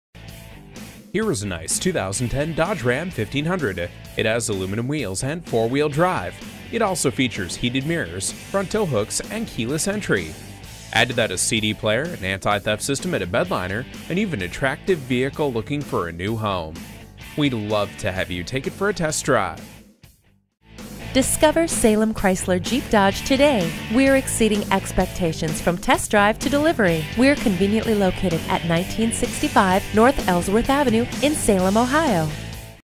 2010 Dodge Ram 1500 Truck in Salem Alliance, OH 44460 - SOLD
SOLD SOLD - Take a look at this new 2010 Dodge Ram 1500 Truck, Stock: 102. Salem Chrysler Jeep Dodge 1965 North Ellsworth Ave Salem Alliance OH, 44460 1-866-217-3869 Salem Chrysler Jeep Dodge - serving...